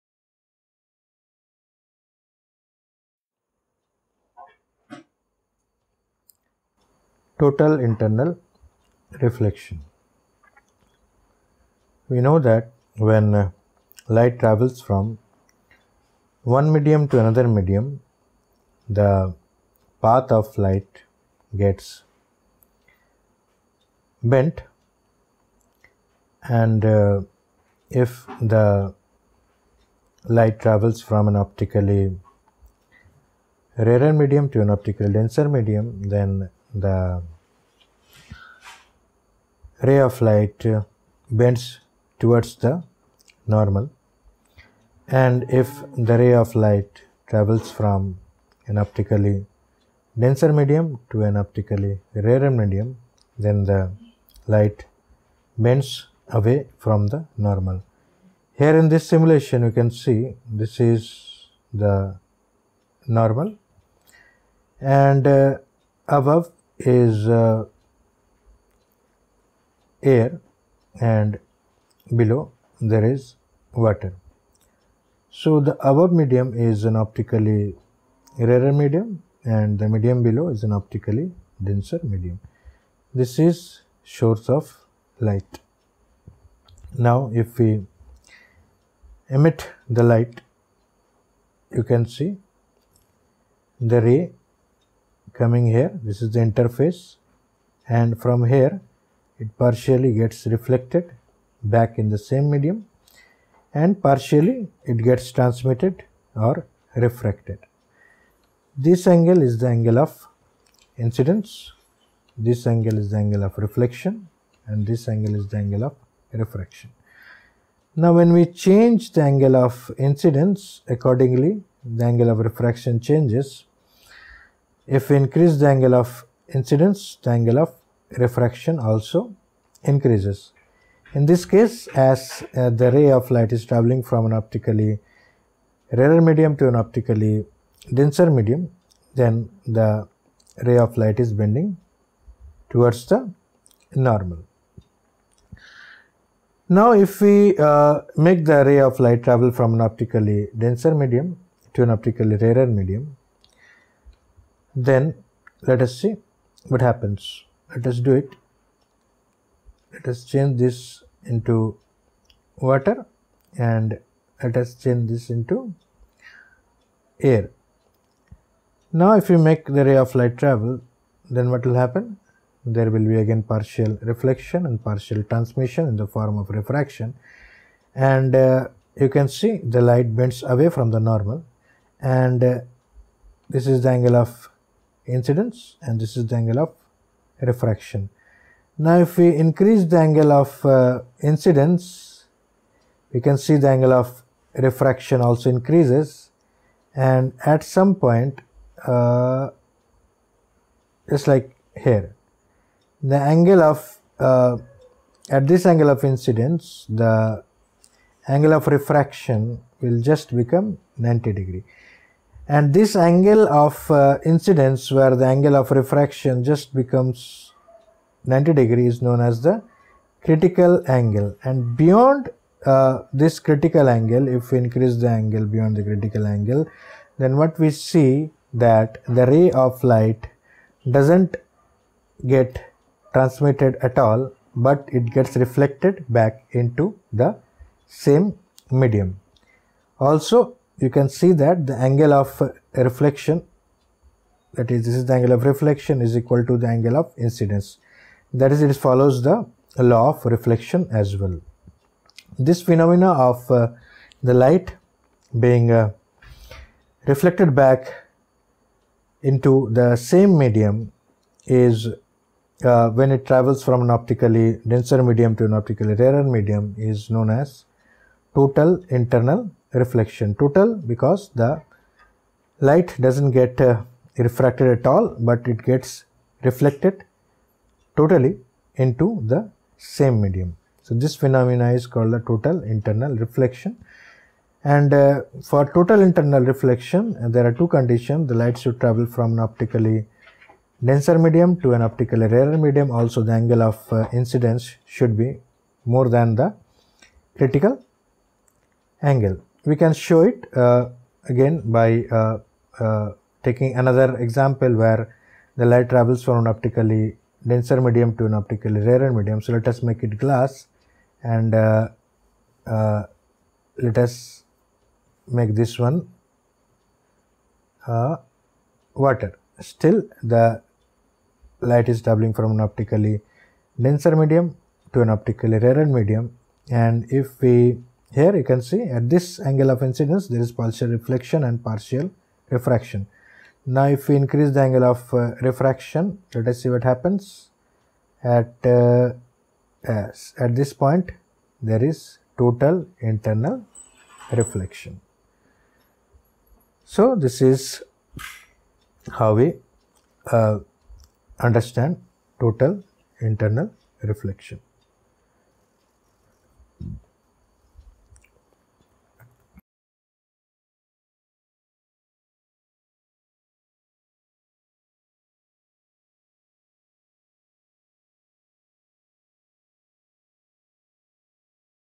Total Internal Reflection
In this short video, we learn the concept of Total Internal Reflection of Light